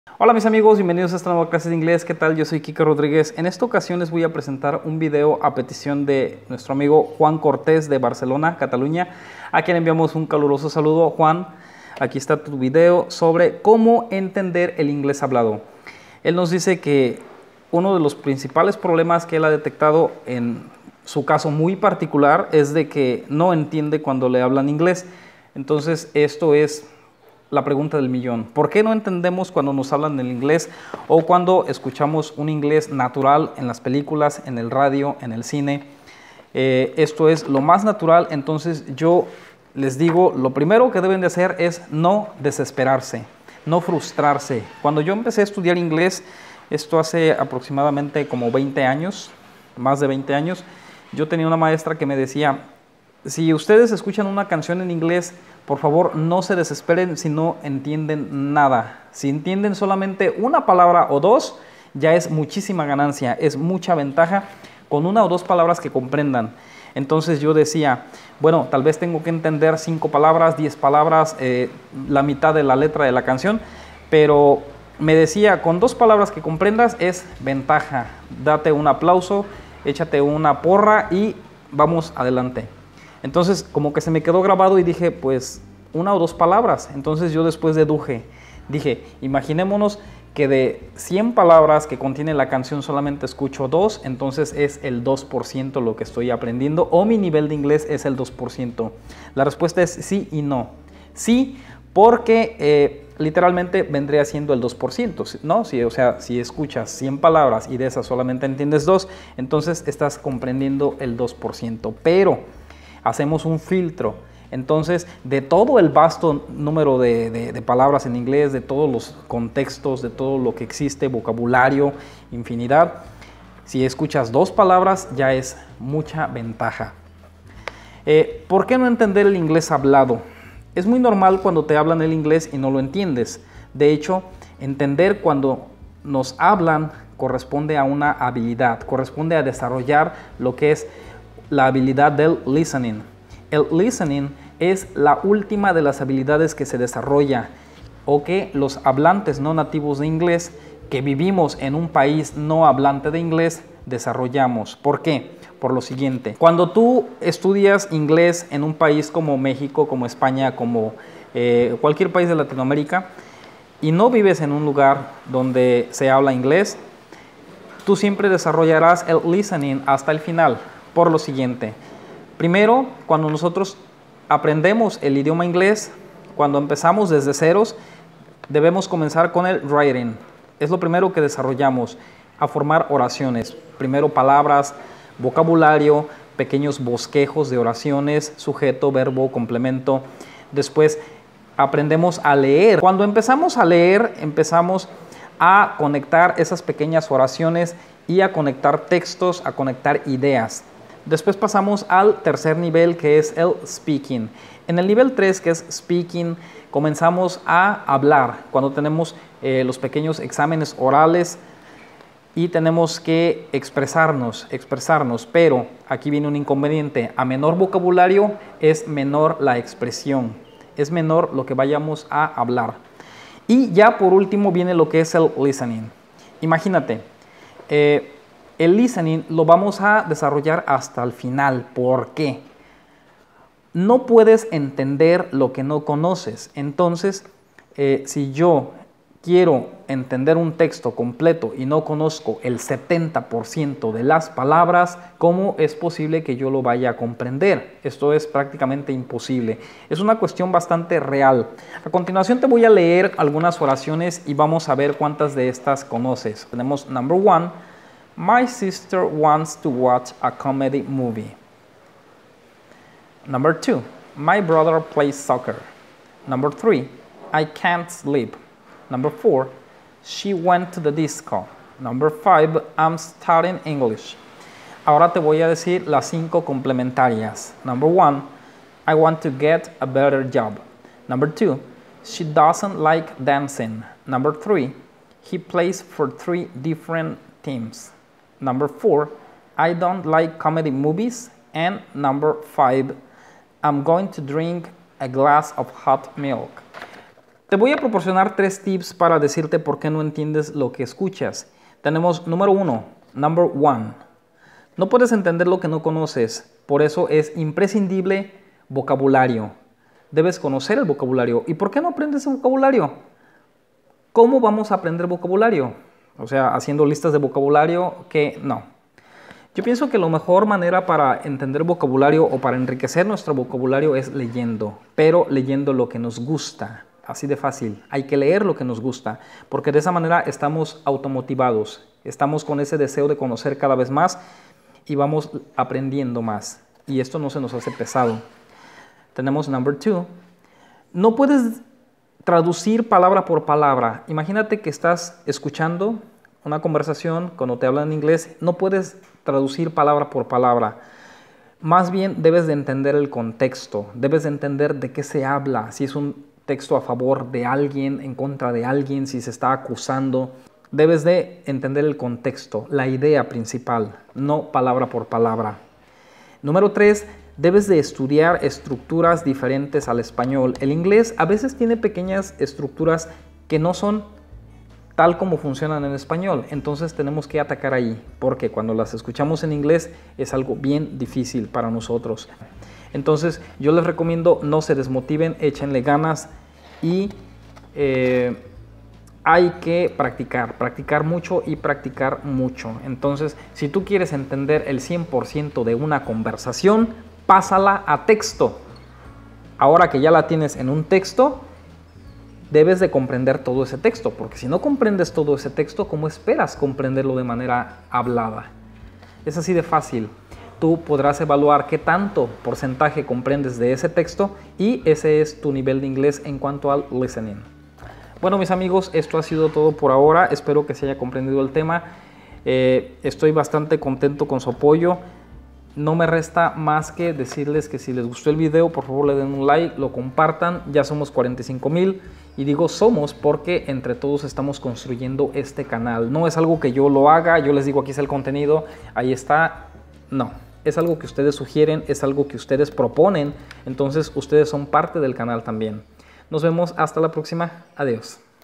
COMO ENTENDER EL INGLES HABLADO (English Listening)
Es la pregunta que mucha gente se hace: COMO entender el INGLES HABLADO (English Listening); Por qué no entiendo cuando hablan INGLES...En este video explico los detalles.